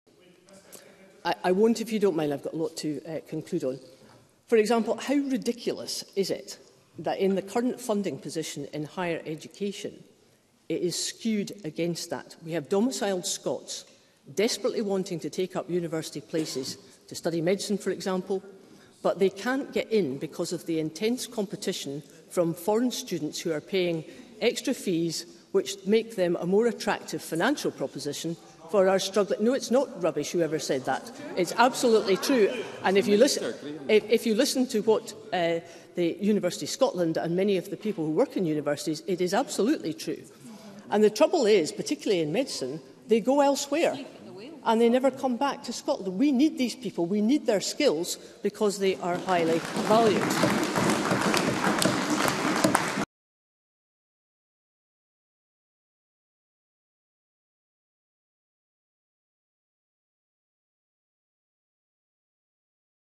Liz Smith shuts down SNP heckler with 'It's not rubbish' blast
At Holyrood this week Liz Smith was talking about Scottish higher education, and how it was harder for Scottish students to get a university place due to foreign students who pay higher fees.

She was heckled by the SNP's minister for independence, Jamie Hepburn.

Ms Smith quick delt with the heckler by dishing out some facts.

Ms Smith said: 'For example, Scotland-domiciled people desperately want to take up university places to study medicine, but they cannot get in because of the intense competition from foreign students who pay extra fees, which makes them a more attractive financial proposition for our struggling...'

Mr Hepburn shouts 'That's rubbish'.

Ms Smith replies: 'No, it is not rubbish. It is absolutely true. If we listen to Universities Scotland and many of the people who work in universities, we will hear that it is absolutely true. The trouble is that, particularly in relation to medicine, people go elsewhere and never come back to Scotland. We need those people and their skills, because they are highly valued'.